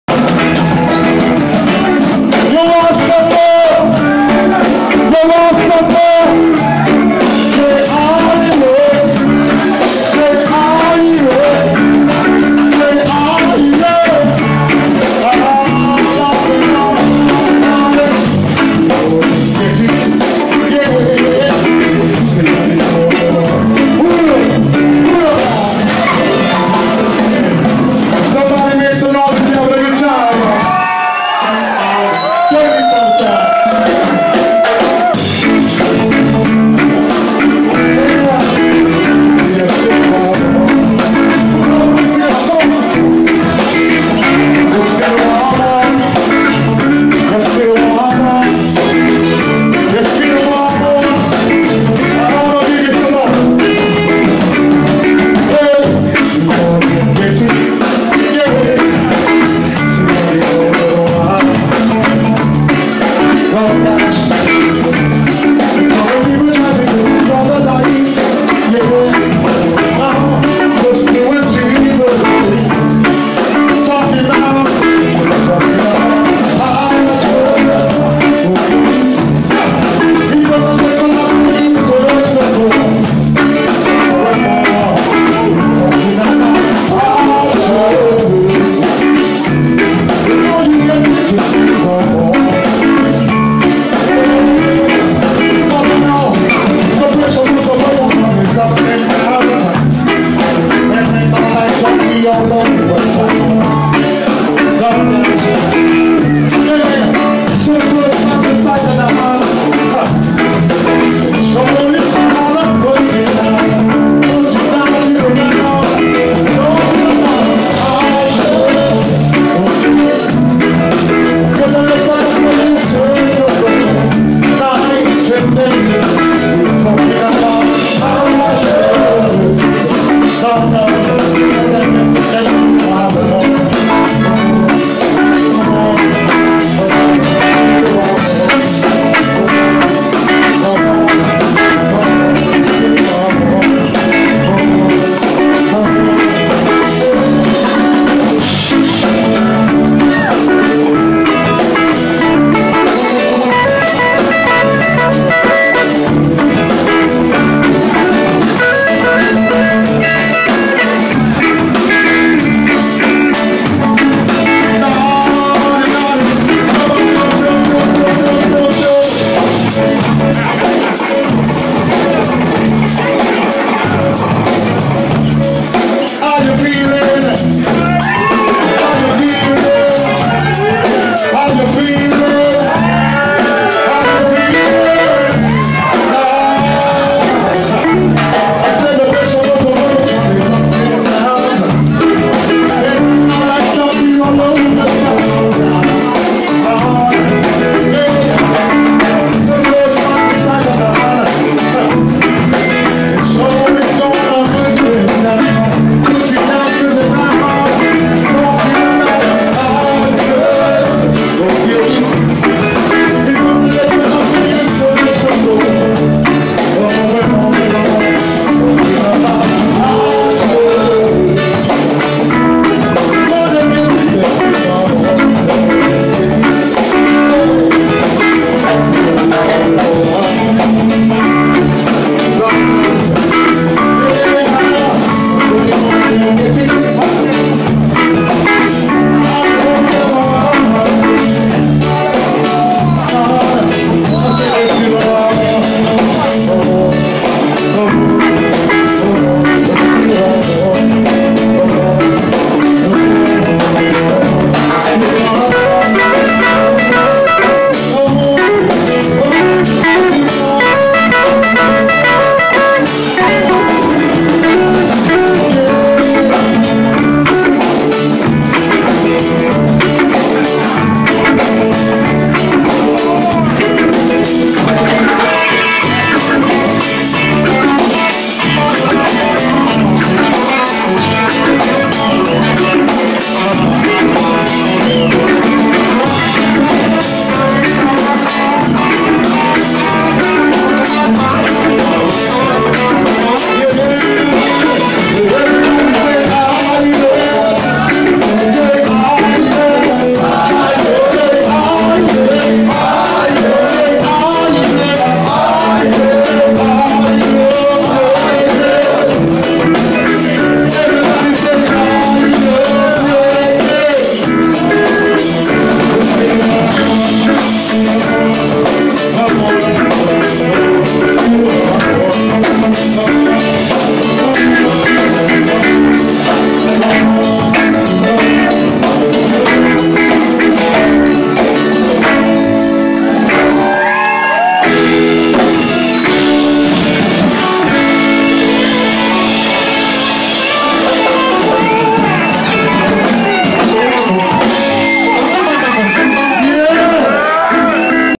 Fully Fullwood Entertainment "Fighting Poverty by Reggae Music for the People!"
This presentation is to help save America's youth on the streets from homelessness!

Fullyfulwood (Base)
Errol Bonnick (Singer)
Jelanie Jones (Piano)
Tony Chin (Guitar)
Rock Deadrick (Drummer)
Bruno Coon (Guitar)

"Much love and God Bless us all"!  May the you be our future and our only concern!

United Intellectual Property.com "Rastas on the Loose"